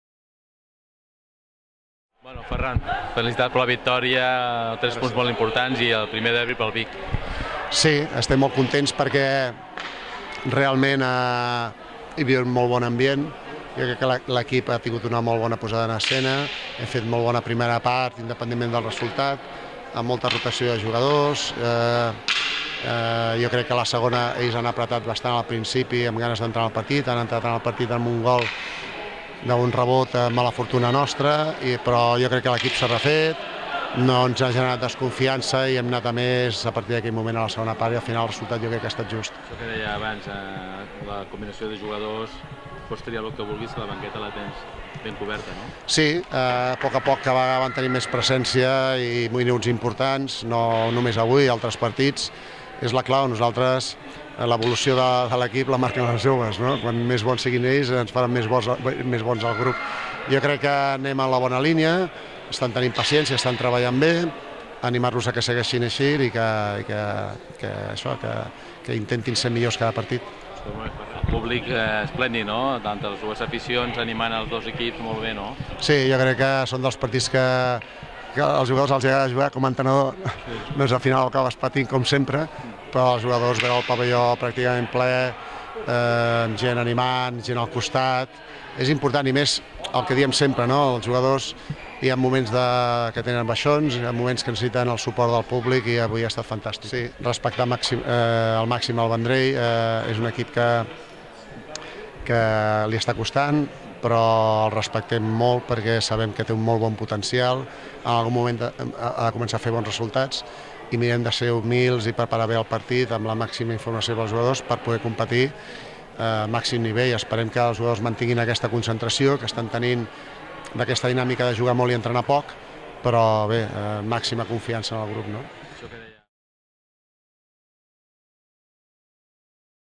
141108 declaracions d @fpujalte @clubpativic 4-1 @cpmanlleu oklliga @Vigatans1705 @el9nou
Declaracions de l'entrendador del CP Vic Ferran Pujalte postparit del derbi entre el CP Vic i CP Manlleu amb el resultat final de 4 a 1 pel Vic.